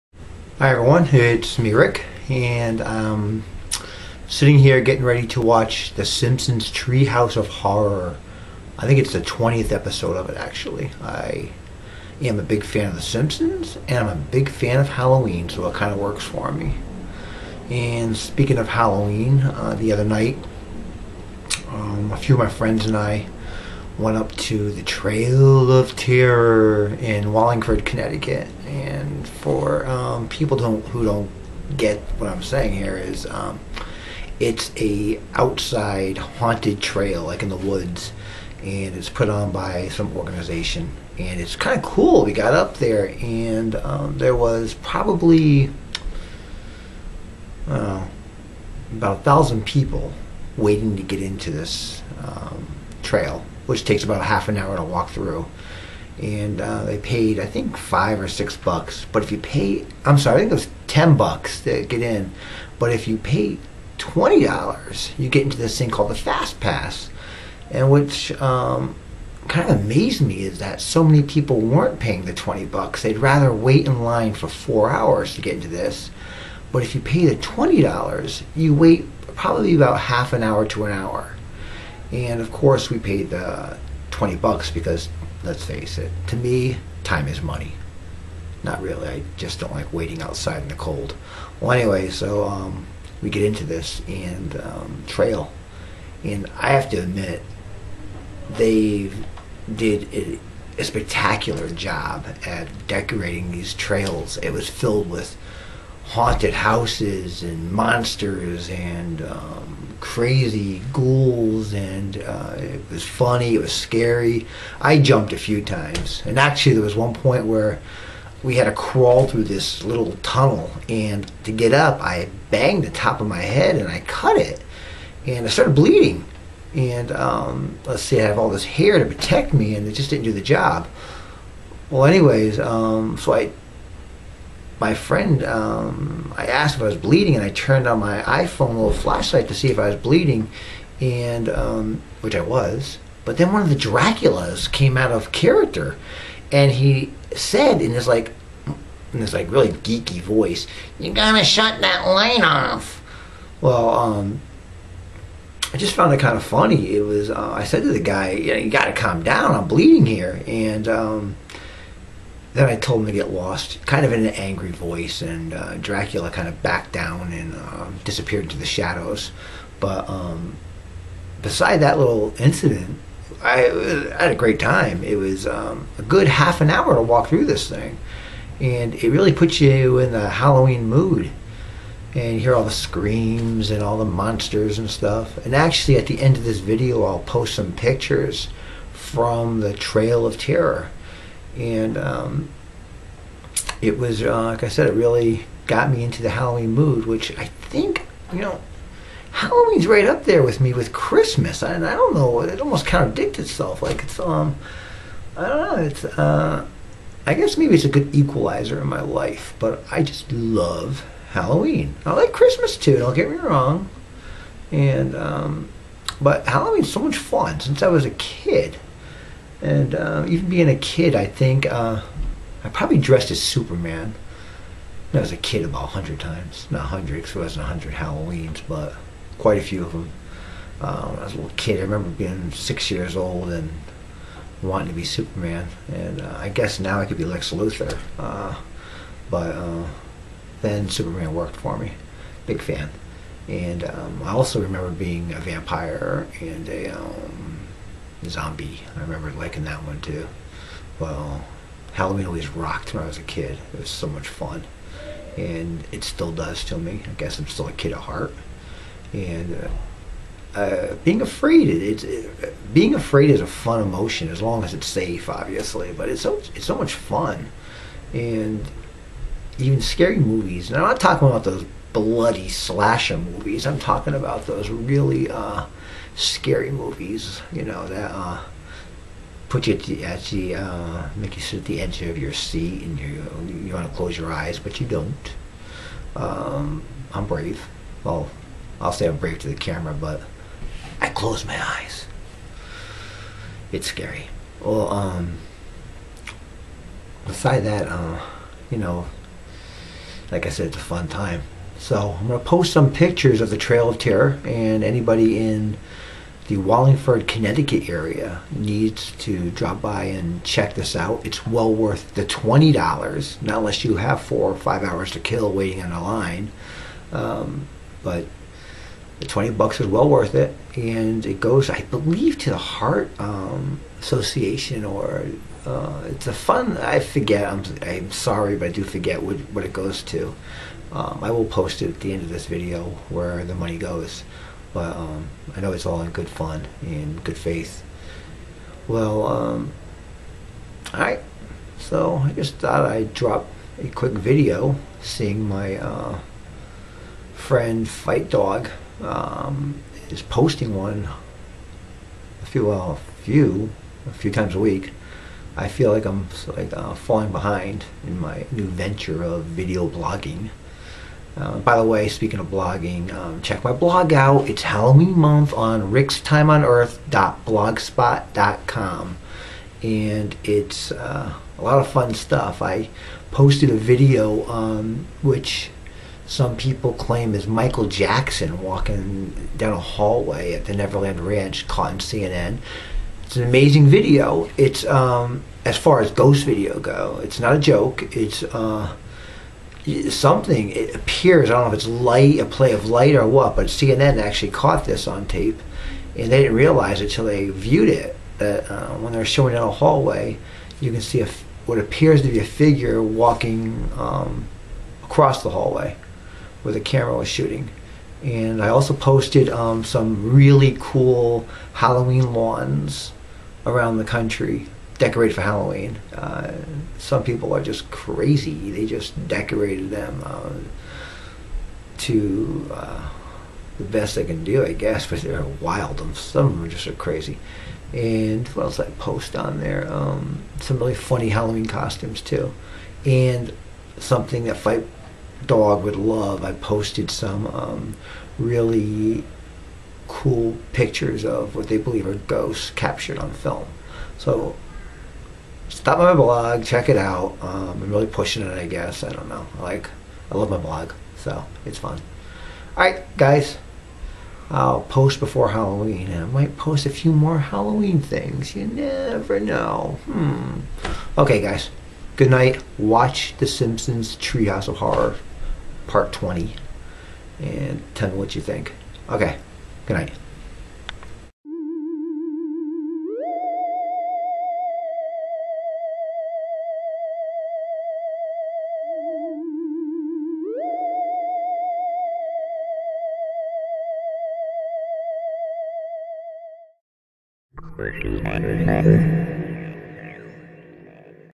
The Trail Of Terror
Some Halloween fun on The Trail Of Terror in Wallingford, CT